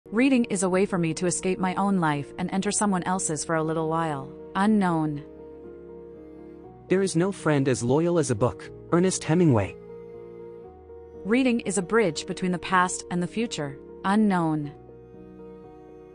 Uncover Hemingway's Views on Reading: You'll Be Surprised!
We've collected some of the best motivational quotes about reading and books. Such as:

"Reading is a way for me to escape my own life and enter someone else's for a little while."  ~Unknown

"There is no friend as loyal as a book."  ~Ernest Hemingway

"Reading is a bridge between the past and the future."  ~Unknown